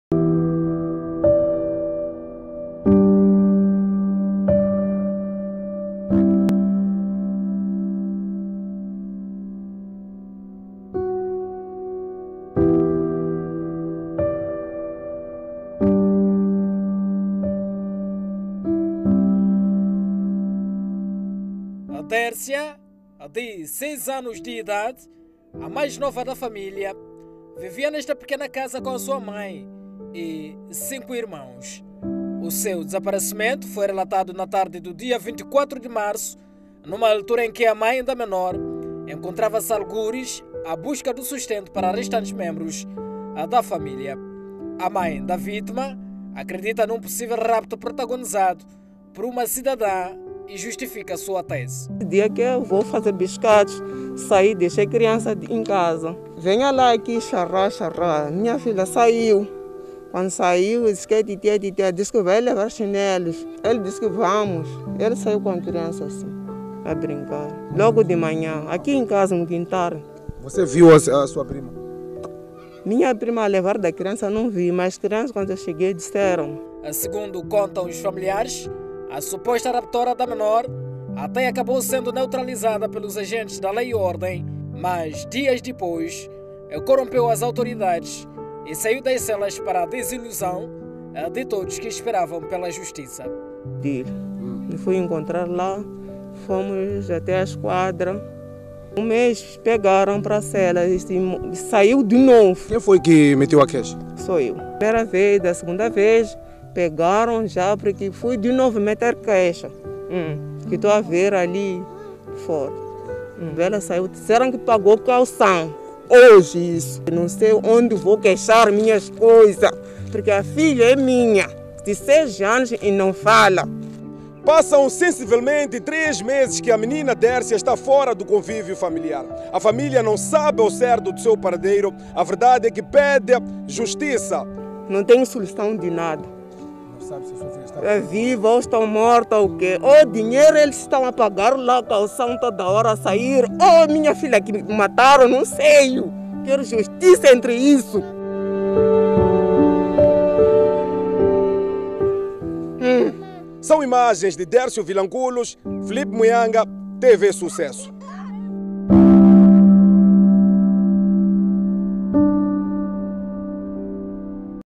Menor desaparecida na Matola
CRIANÇA DE SEIS MESES DESAPARECE MISTERIOSAMENTE NO BAIRRO INTAKA E SUSPEITA-SE QUE TENHA SIDO RAPTADA